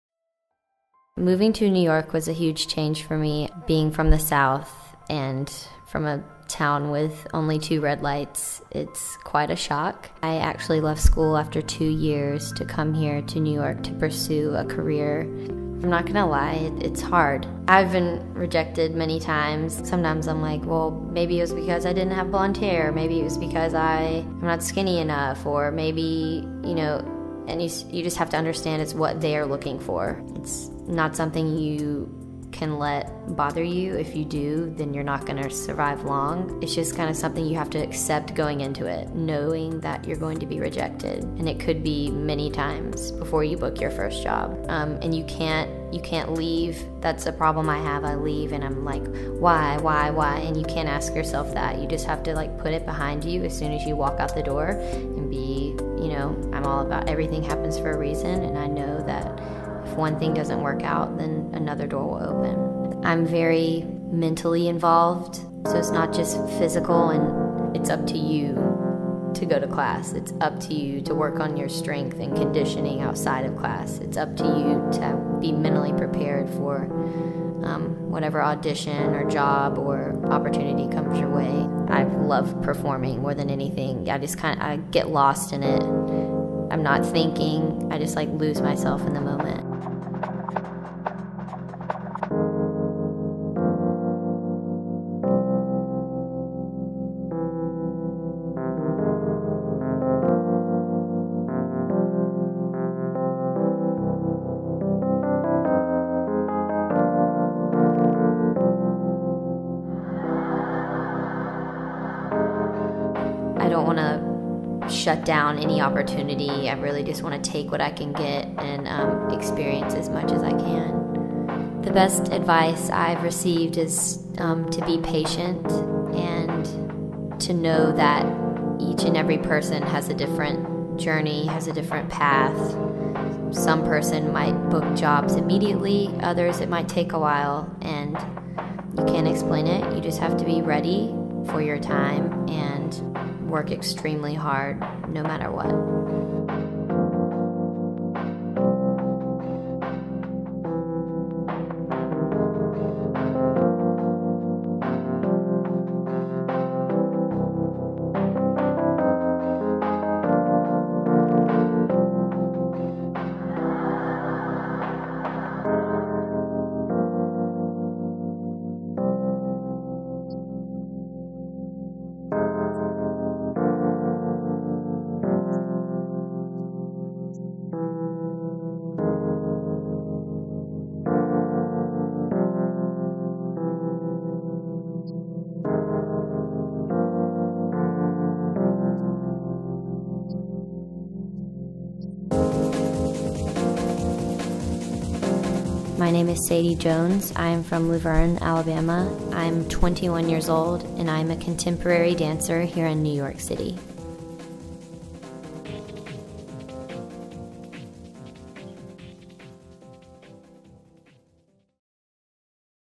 Sadie Jones - Contemporary Dancer NYC
Portraits and interview of Sadie Jones whose a 21 year old Contemporary Dancer located in NYC.

Written, Director and photographed by Alexis Cuarezma